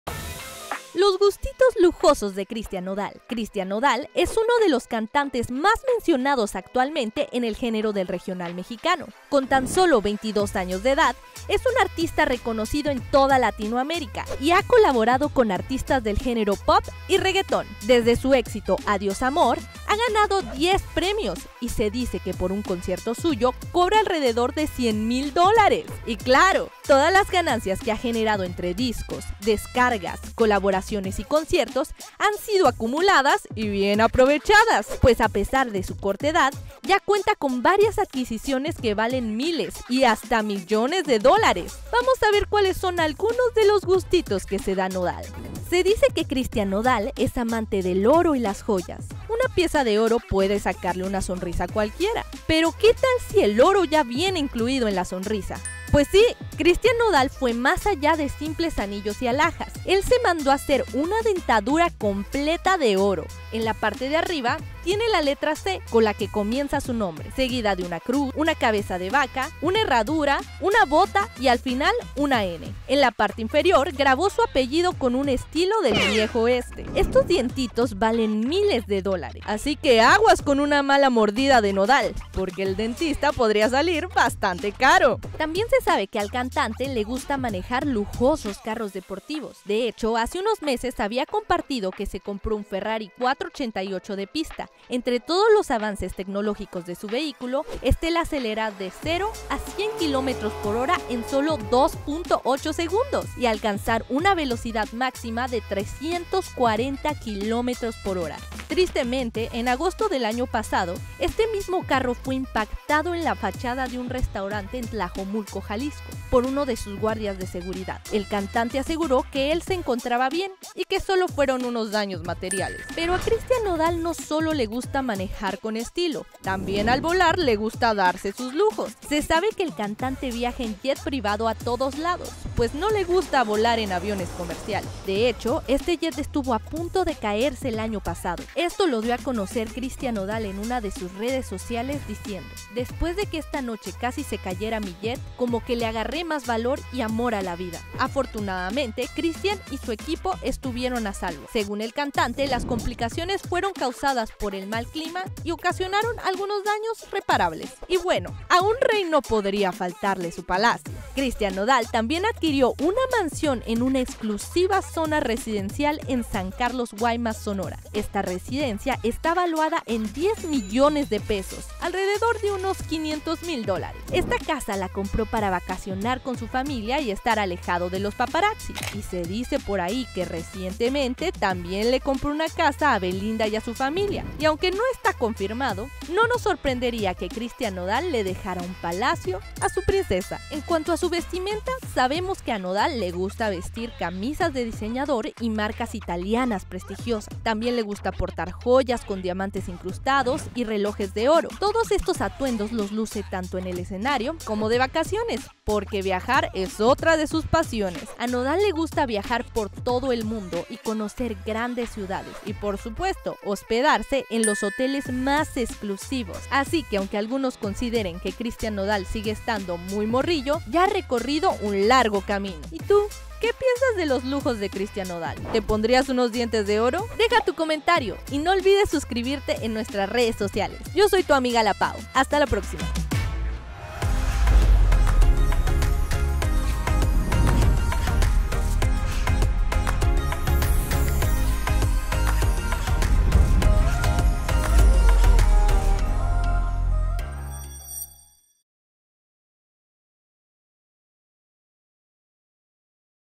Christian NODAL y sus pequeños LUJOS | EstrellaTV
A su corta edad es uno de los músicos mas reconocidos y gracias a ello ha logrado una creciente fortuna.